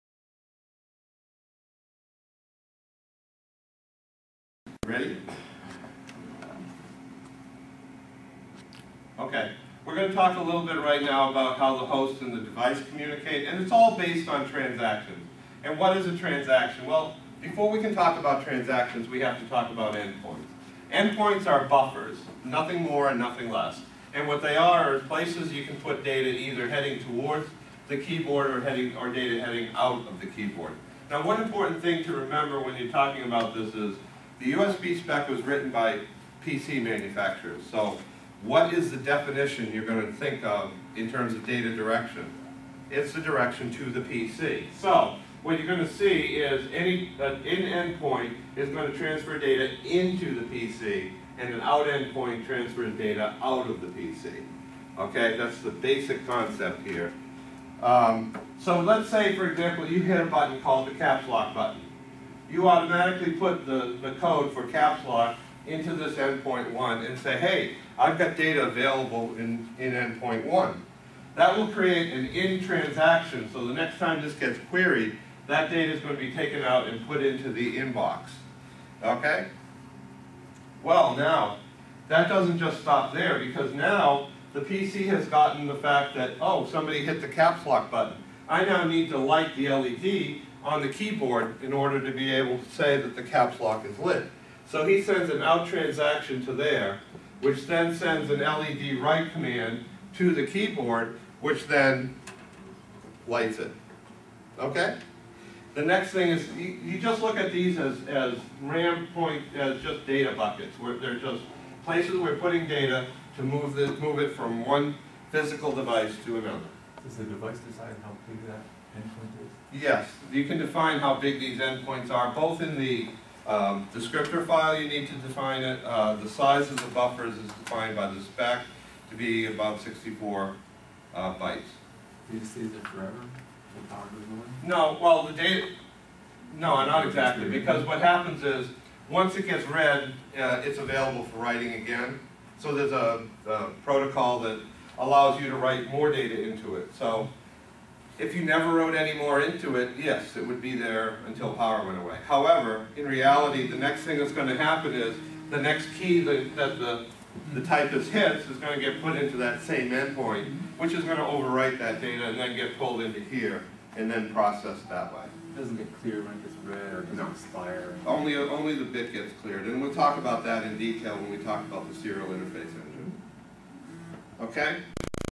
Microchip's Regional Training Center, USB Class
This is a 3 minute clip from one of Microchip's Regional Training Center classes on USB.  Microchip also offers many other classes (i.e. Motor Control, Ethernet, Embedded C Programming, etc...) for Electrical Engineers.  to find a class in your area.